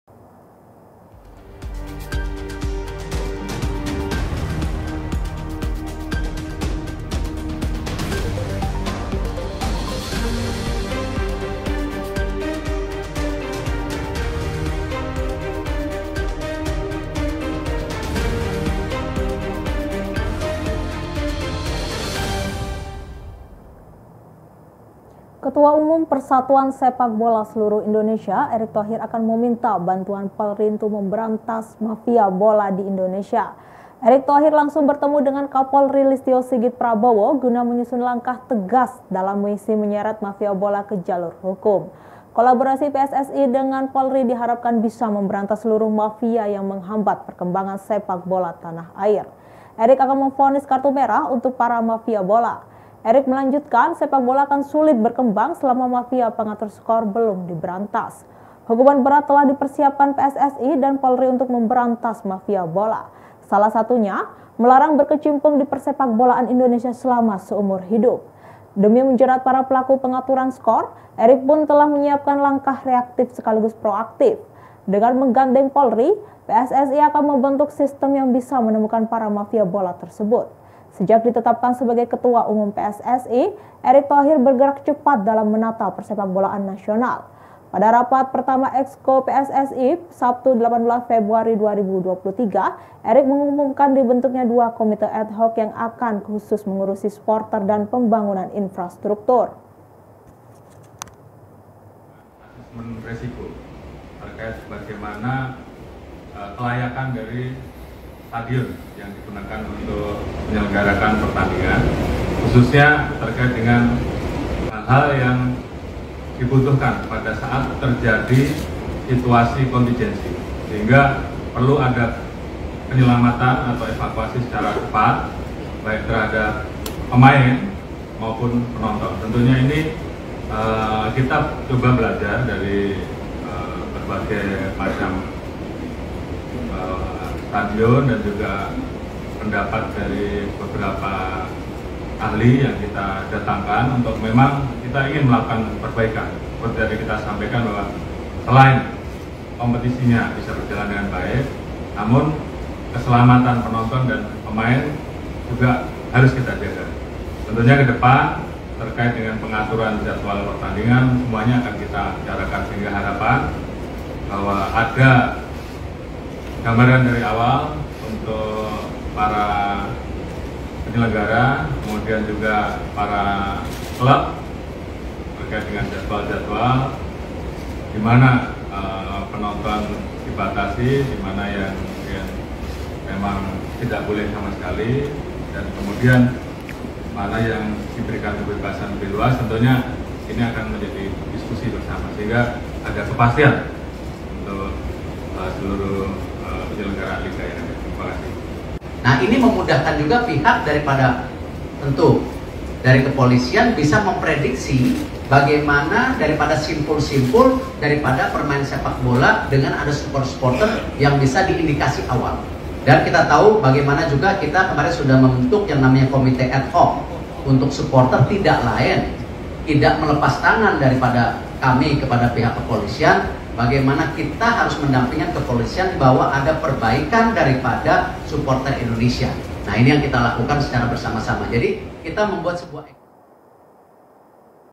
Cara Erick Thohir Basmi Mafia Bola, Minta Bantuan Polri dan Siapkan Hukuman Berat
Ketua Umum (Ketum) PSSI, Erick Thohir akan meminta bantuan Polri untuk memberantas mafia bola di Indonesia.

Erick Thohir langsung bertemu dengan Kapolri Listyo Sigit Prabowo guna menyusun langkah tegas dalam misi menyeret mafia bola ke jalur hukum.

Kolaborasi PSSI dengan Polri diharapkan bisa memberantas seluruh mafia yang menghambat perkembangan sepak bola tanah air.

Artikel ini telah tayang di Tribunnews.com dengan judul Cara Erick Thohir Basmi Mafia Bola, Minta Bantuan Polri dan Siapkan Hukuman Berat,
Penulis: Rochmat Purnomo